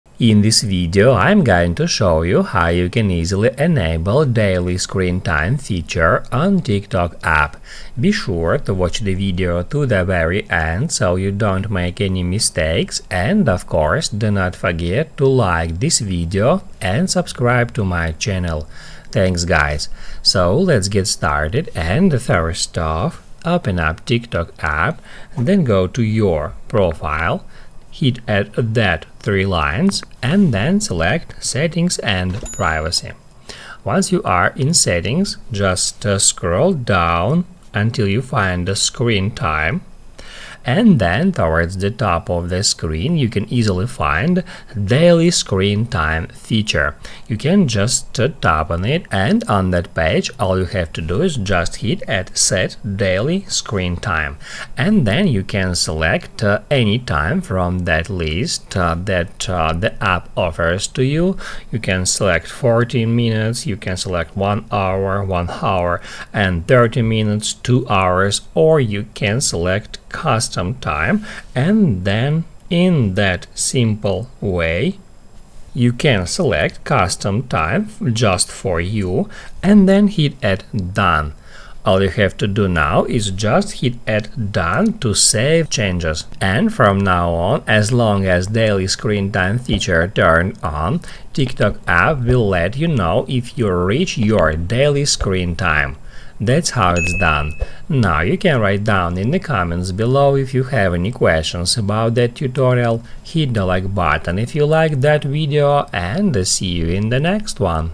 How to Set Daily Screen Time on Tiktok - Full Guide
If you are looking for a video about how to set daily screen time on tiktok, here it is!
You will learn how to limit screen time on tiktok. That's easy and simple to do from your phone.

00:00 - Intro
00:25 - How to set daily screen time on tiktok - full guide